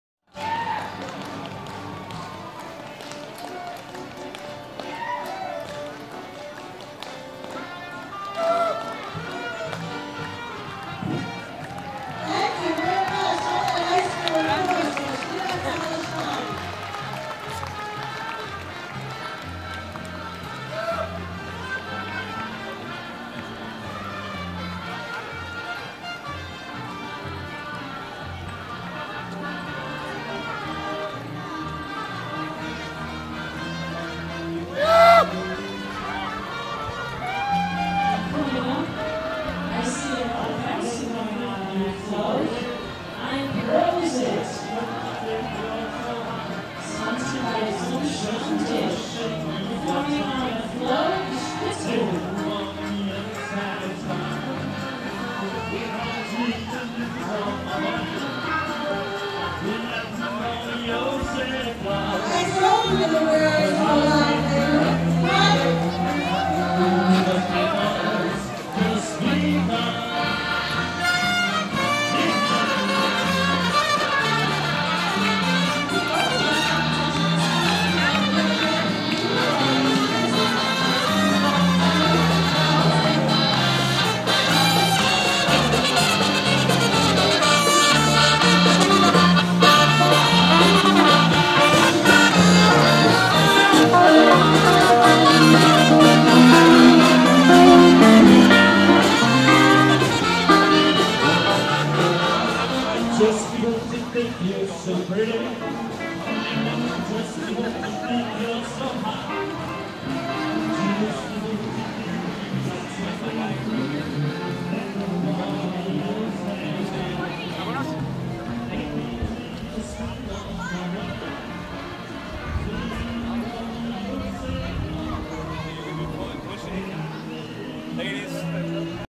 Spitzbuam Band at Steuben Parade 2012
An American Band  that specializes in traditional German music performing on a float at the 2012 .